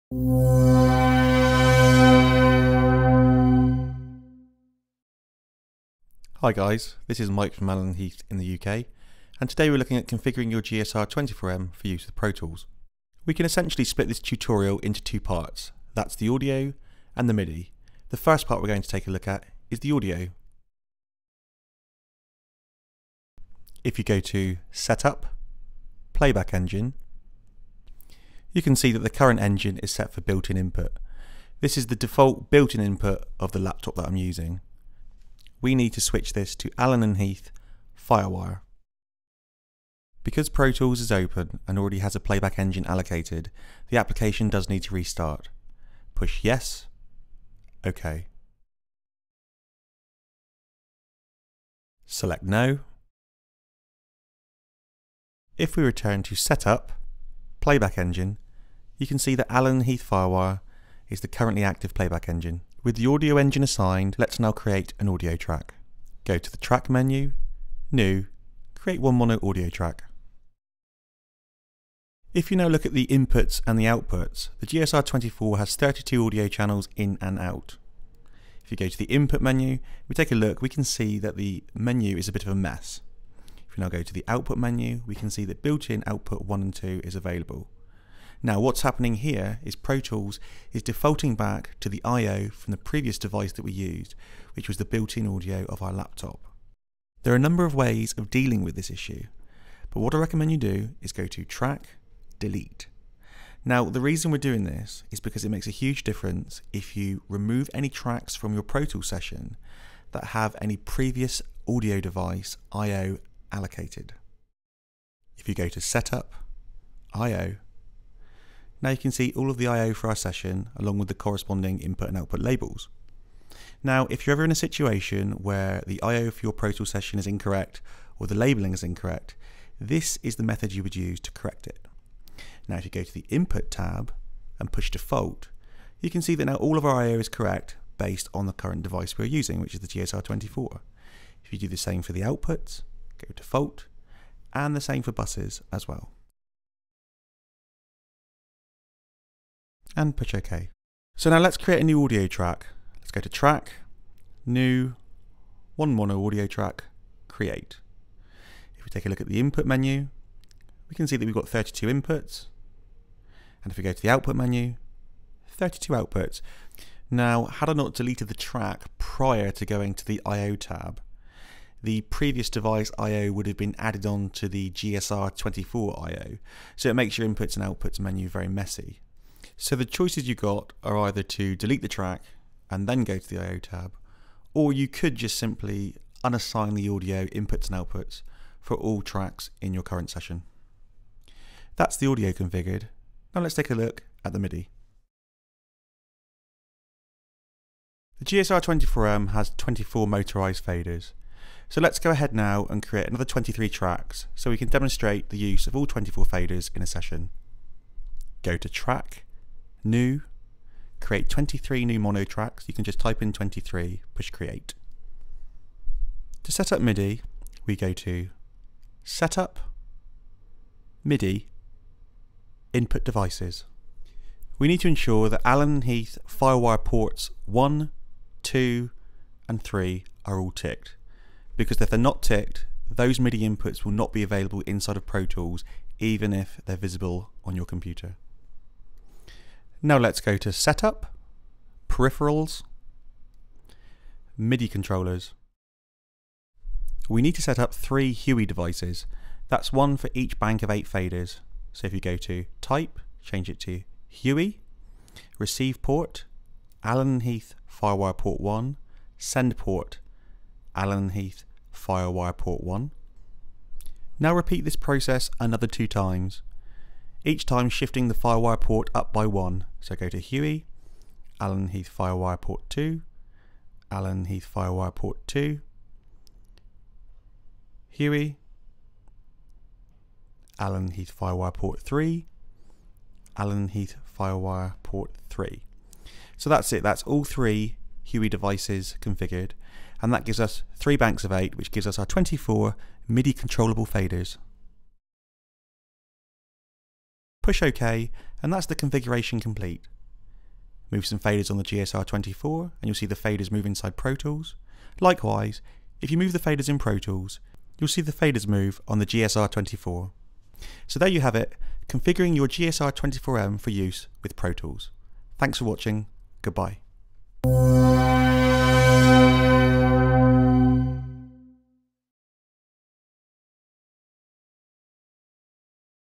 Pro Tools setup with Allen & Heath GS-R24M
Configuring your GS-R24M and firewire module for Pro Tools operation.

Includes audio, I/O and native HUI control surface configuration.

PLEASE NOTE: You must be in MIDI start-up mode 6 for Pro Tools operation.